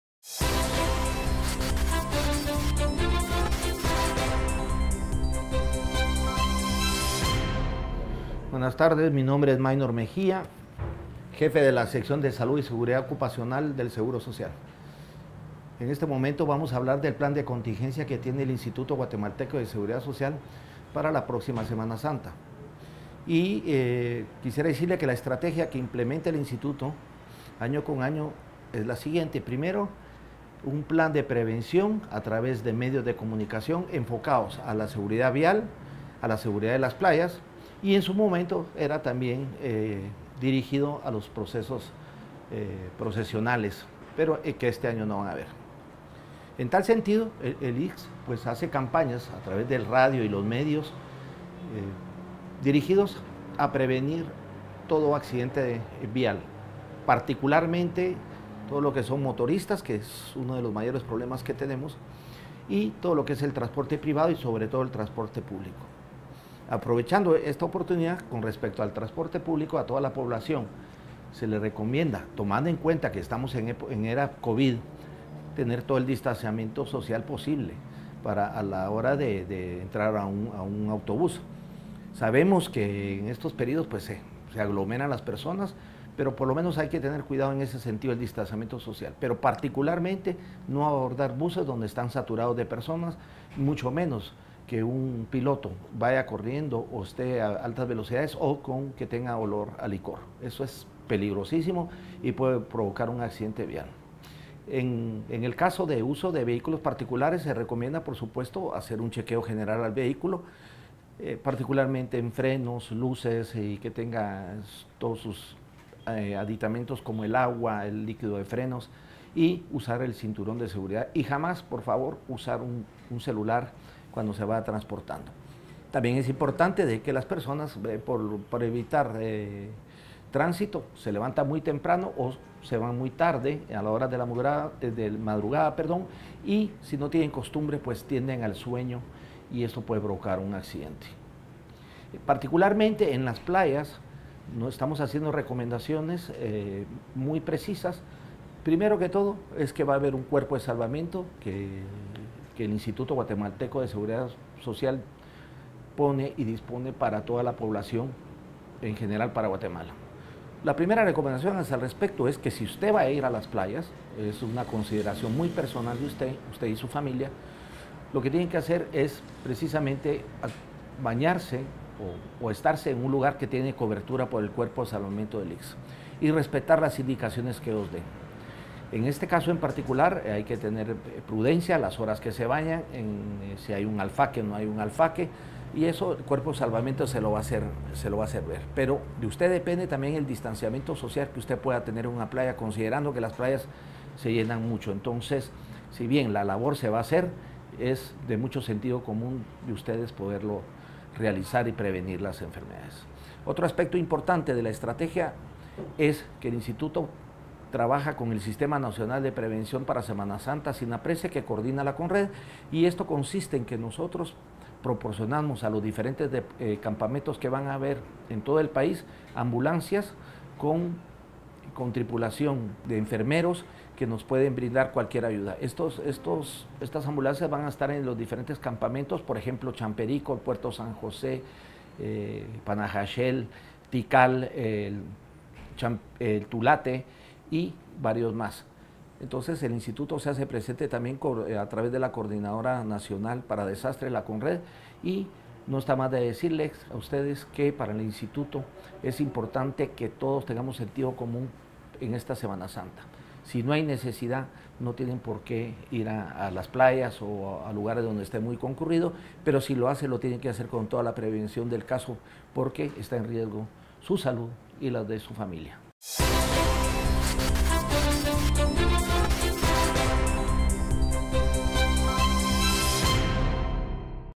Medidas de Prevención durante la Semana Santa, IGSS TV 169
Entrevista al Dr. Mynor Mejía.